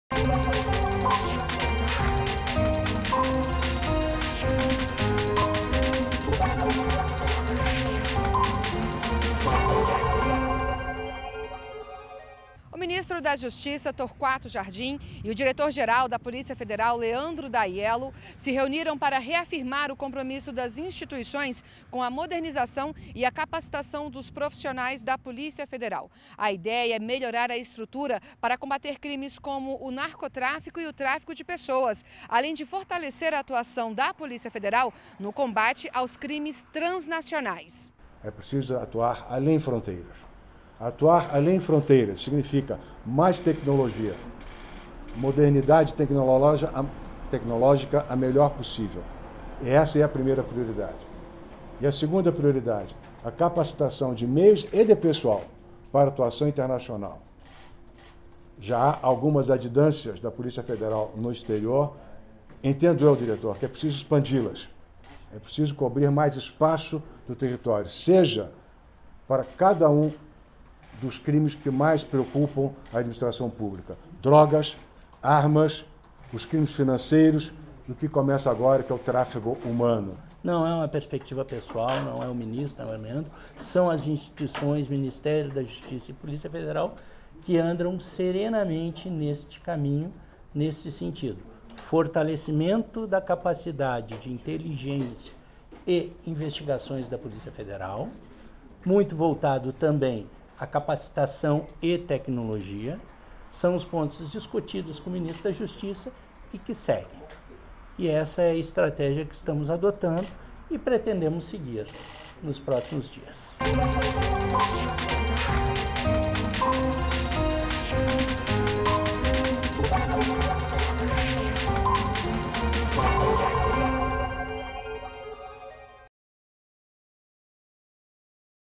Justiça e Polícia Federal reafirmam compromisso com a modernização e capacitação de profissionais
REPÓRTER NBR - 24.06.17: O ministro da Justiça, Torquato Jardim, e o direito-geral da Polícia Federal, Leandro Daiello, se reuniram para reafirmar o compromisso das instituições com a modernização e a capacitação dos profissionais da PF. A ideia é melhorar a estrutura para combater crimes, como o narcotráfico e o tráfico de pessoas.